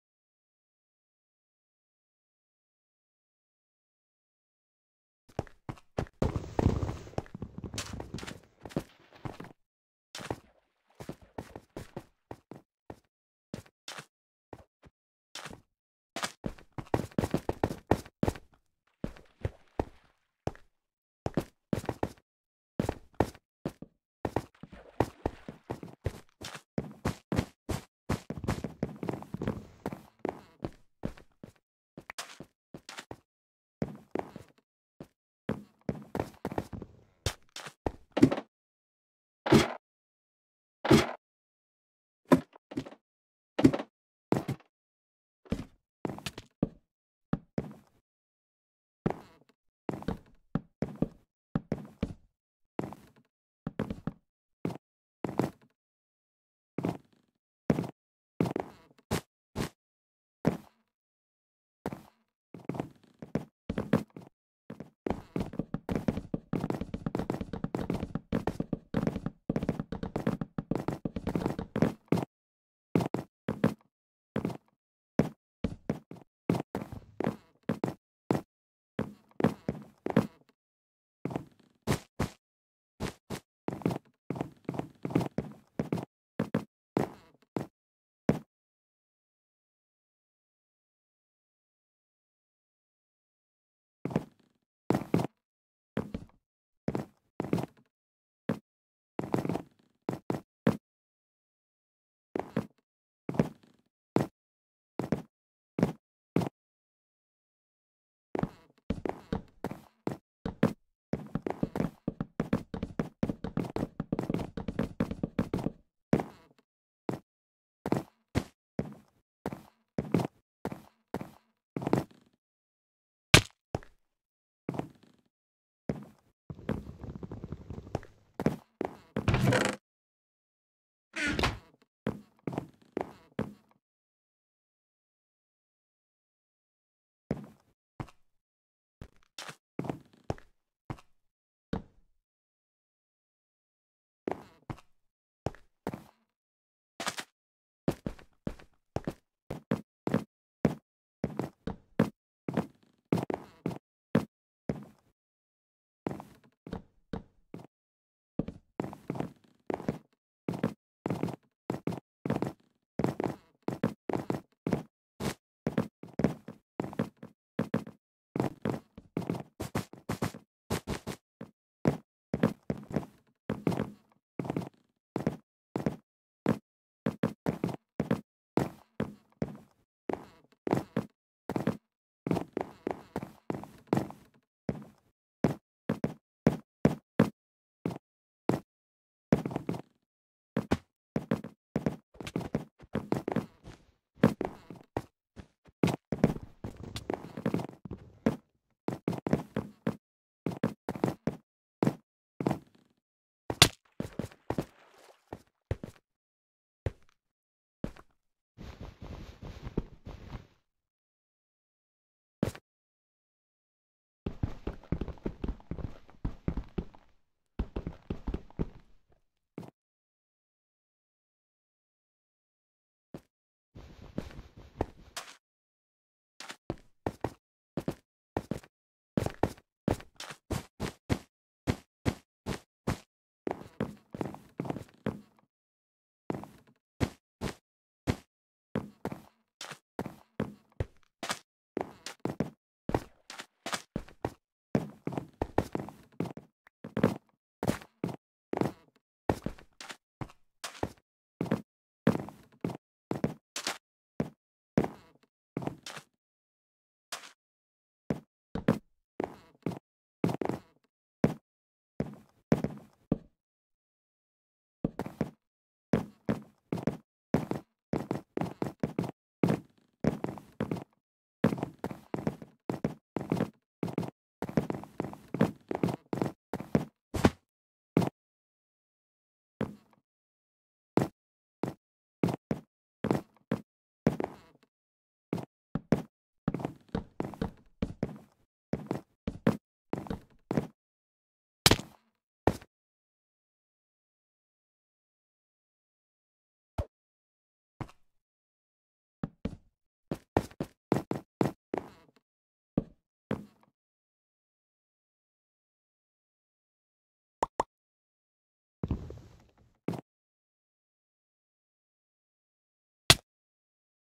Lifeboat CTF is Broken
I went on lifeboat to play some ctf. I joined an origin match, and it was broken. The game wouldn't end because it didn't recognize that the flags were in.